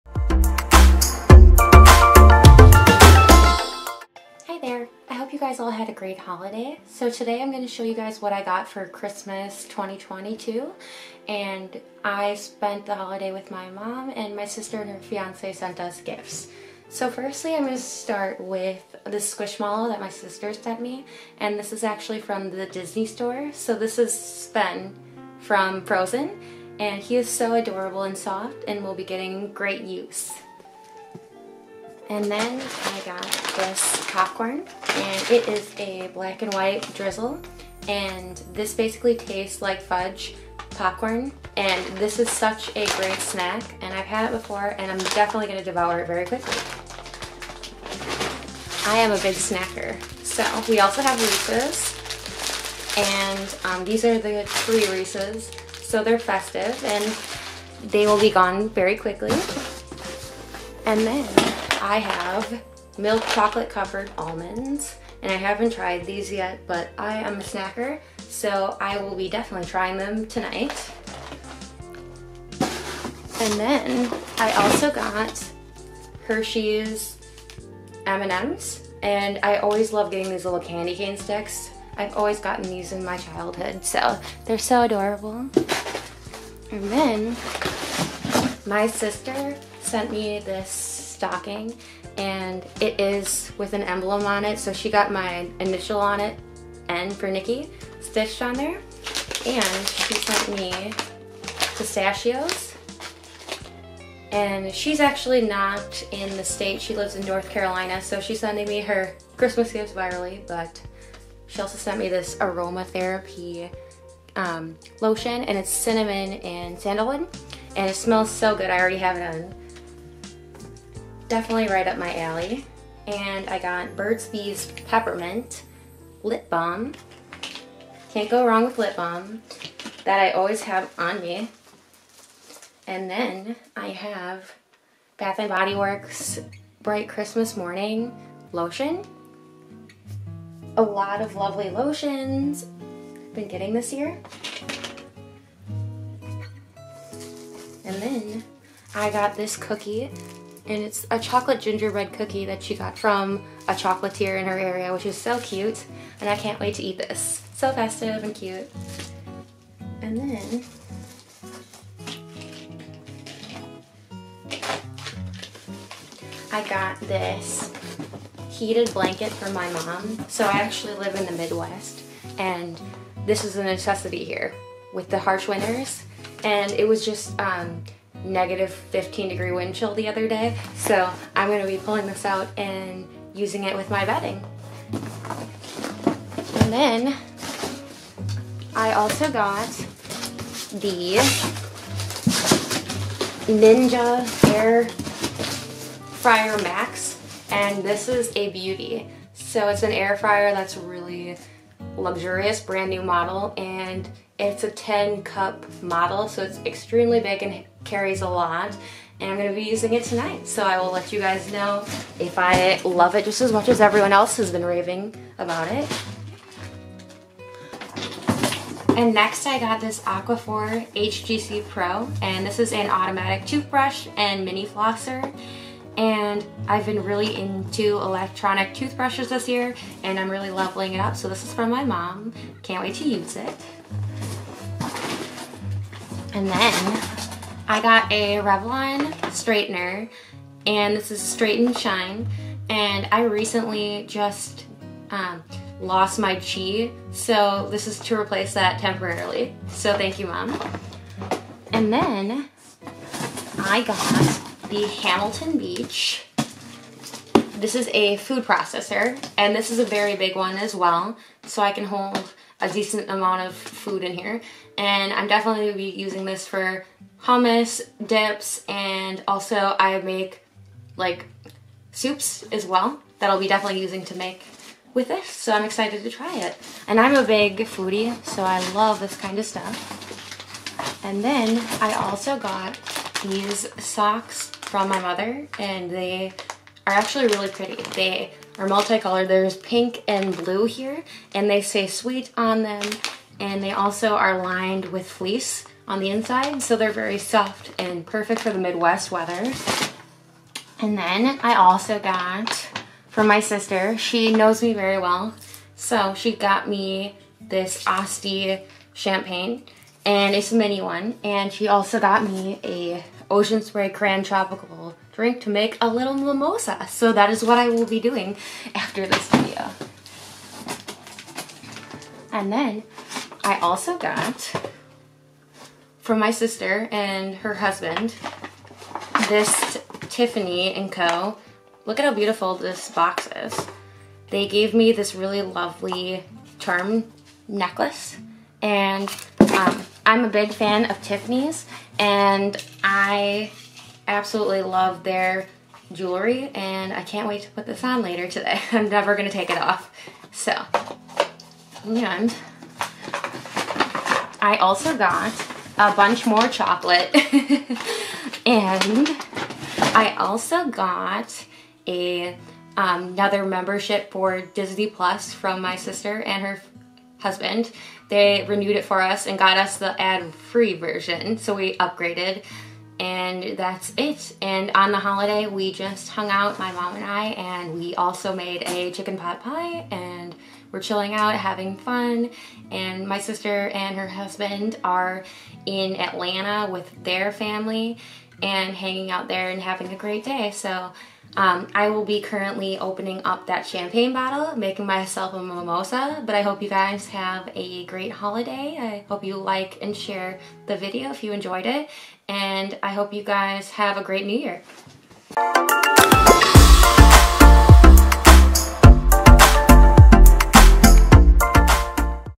What I got for Christmas 2022
Hi lovelies! :)

My name is Nichole! I hope you all had a lovely and safe holiday season! Today I'm going to be showing you the gifts I received for Christmas! :)

xoxo
Nichole